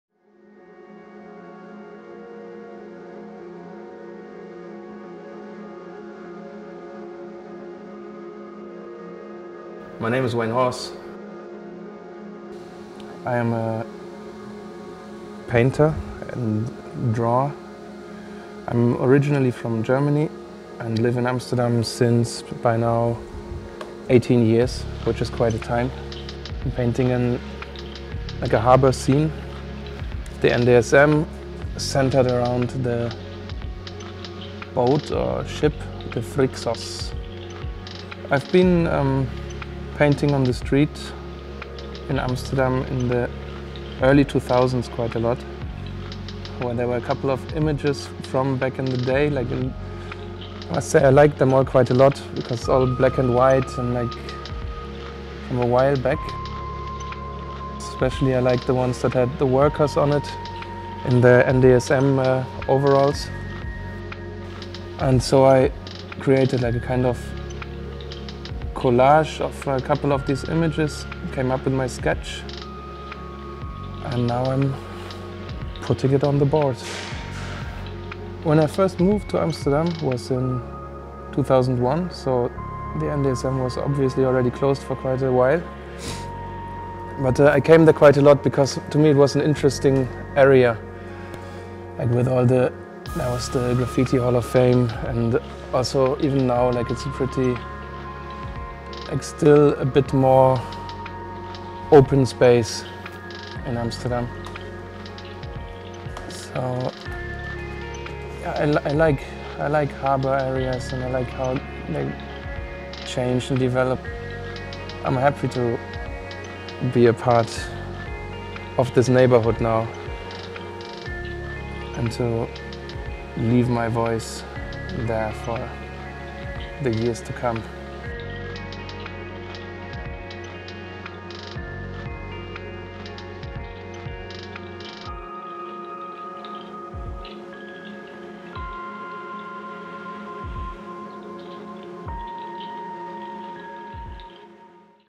Wayne Horse - Frixos
Wayne Horse made a painting for the entrance of the Frixos residential building at former shipyard NDSM in Amsterdam.

An artwork that is part of a project that consists of six murals by six street artists, commissioned by Bouwinvest, curated and produced by Street Art Today.

Filmed and edited by Sandder.